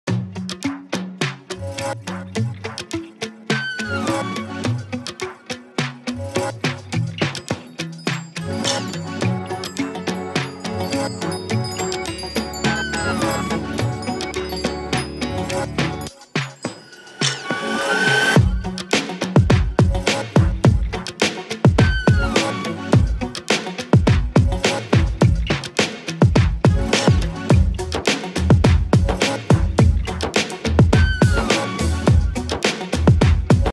C'est pour cela que le volant VIBRE en roulant sur route, les pneus ont été détruits
Conduire ainsi peut être dangereux, ces pneus s'effondrent...